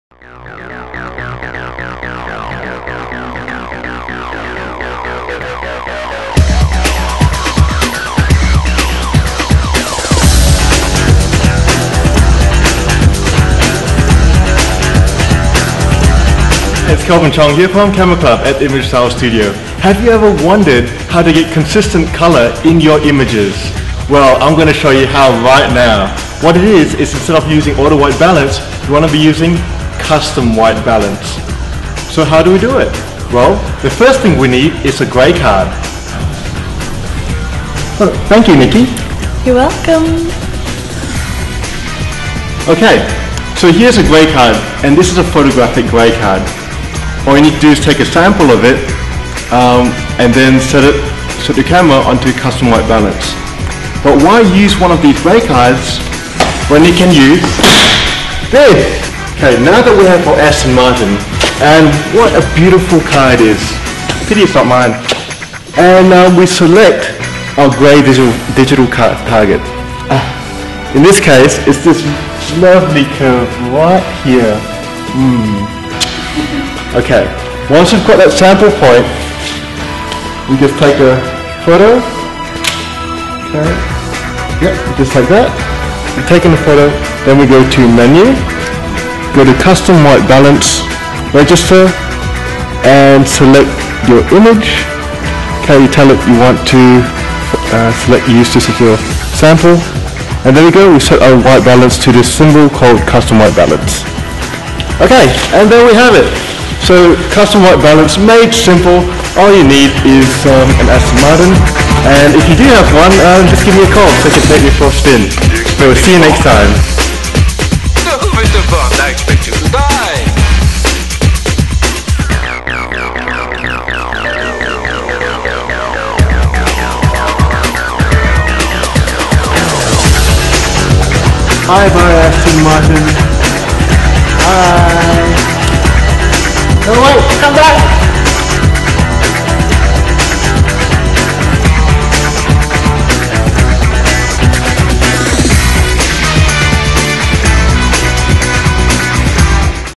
How to Set your Custom White Balance
Achieve accurate colour and consistency in your photos by using Custom White Balance.  You will need a neutral coloured object to sample from, usually known as a photographic grey card or expo disc.